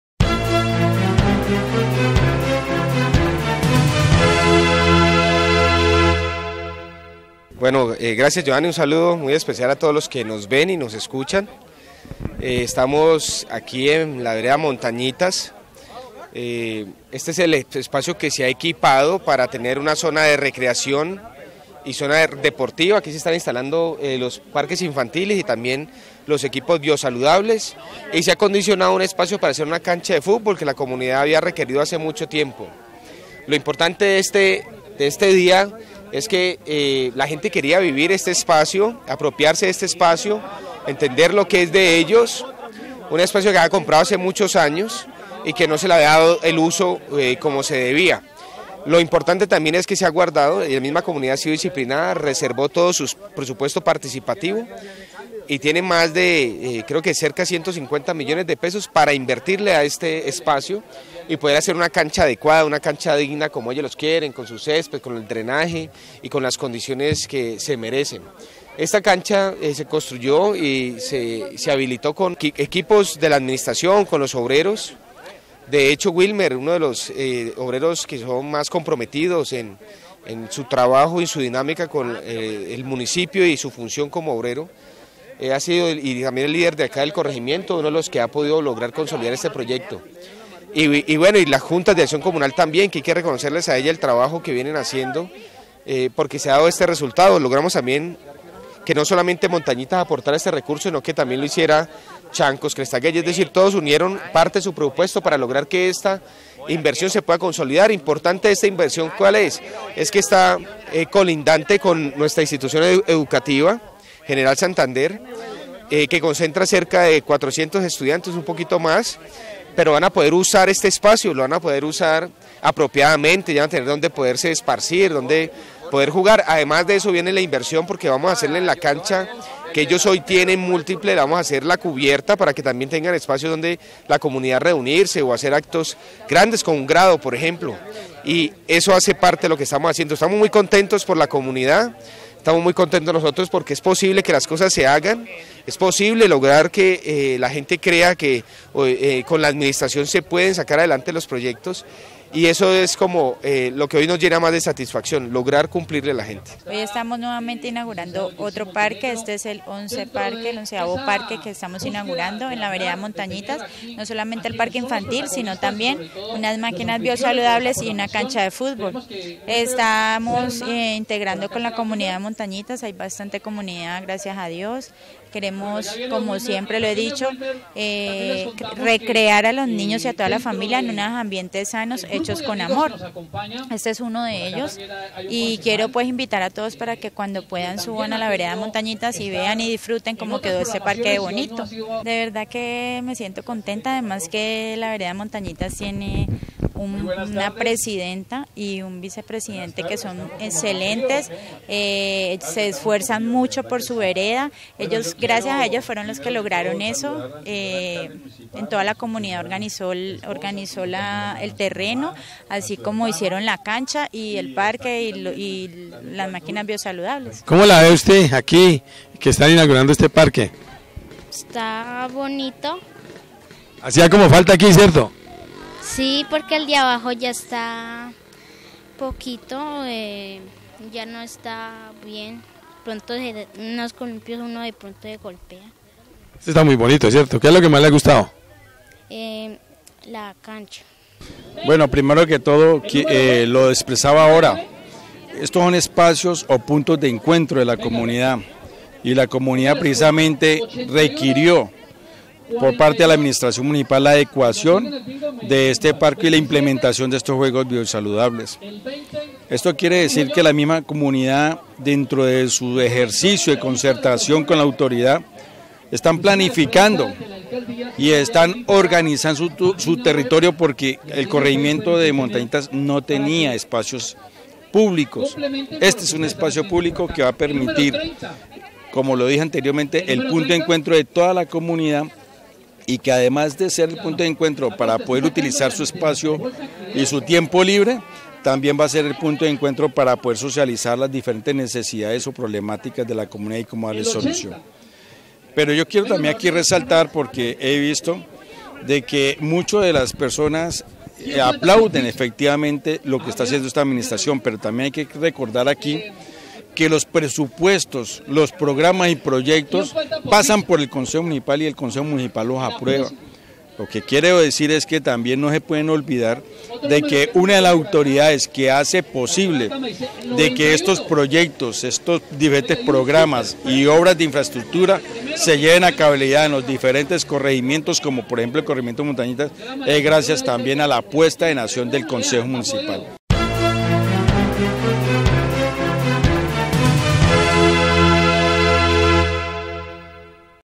INAUGURACIÓN PARQUES INFANTILES- MONTAÑITAS
Se inauguro el parque infantil en la vereda de montañitas.